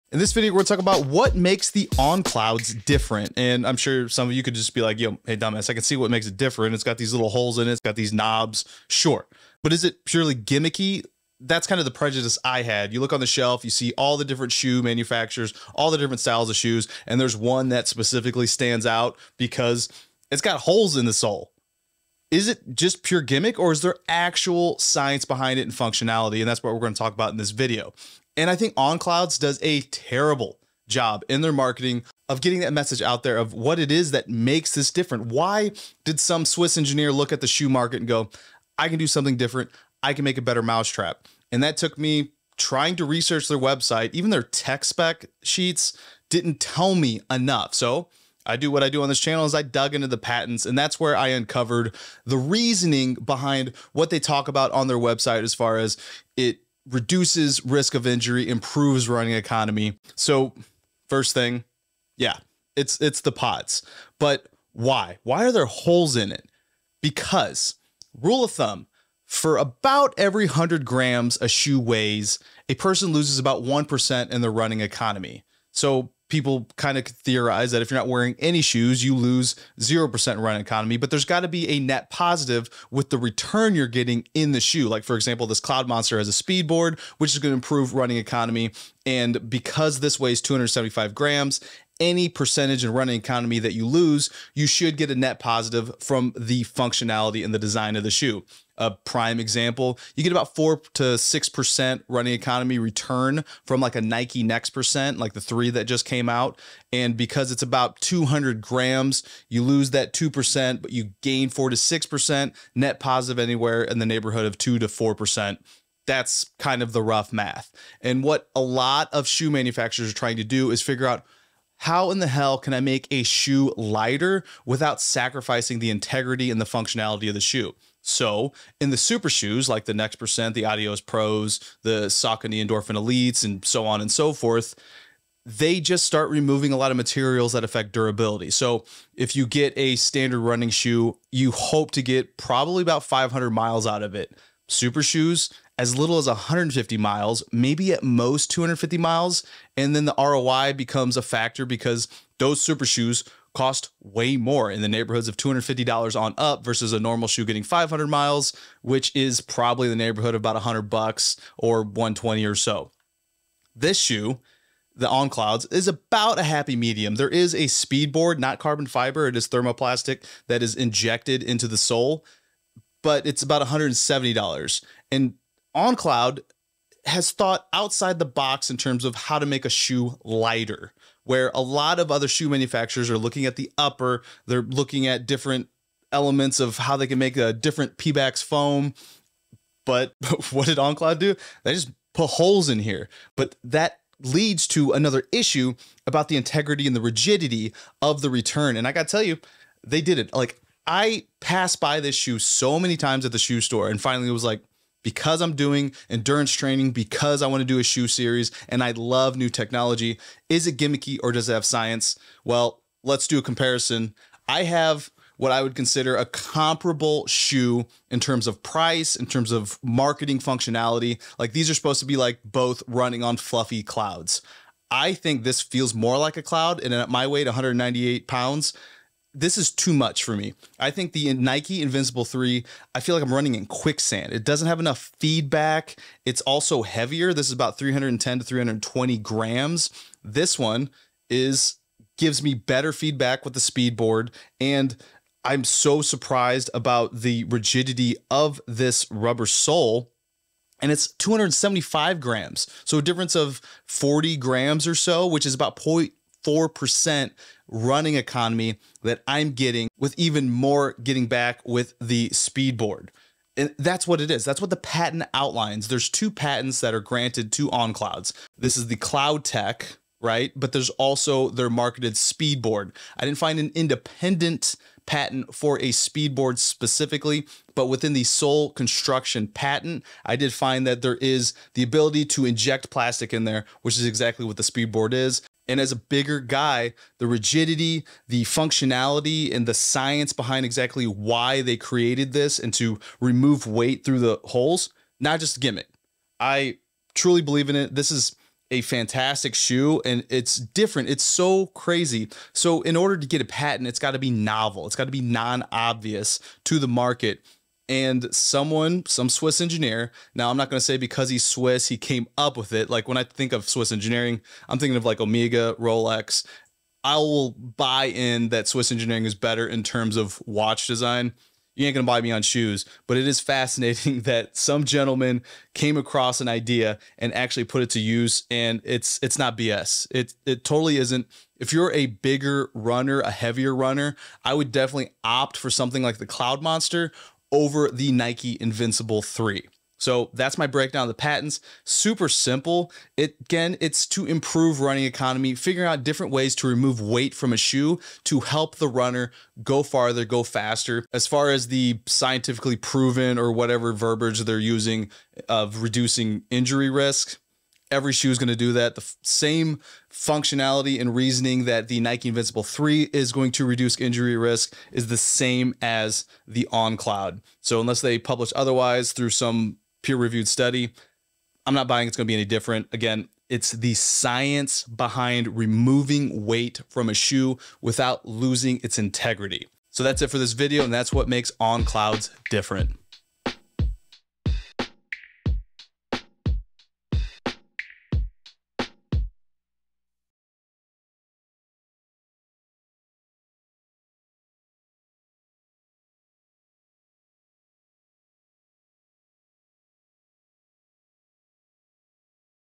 The ACTUAL Science of On Clouds Running Shoes
I've long wondered whether the design of On Clouds running shoes were purely a gimmick. I found that the literature and marketing On Clouds provides didn't satisfy my search to understand the science. So I did what I do on this channel AND, I read the patents. Enjoy

To get your own Eight Sleep mattress or pod pro cover use the following link and code 'WES' for $100 Off at checkout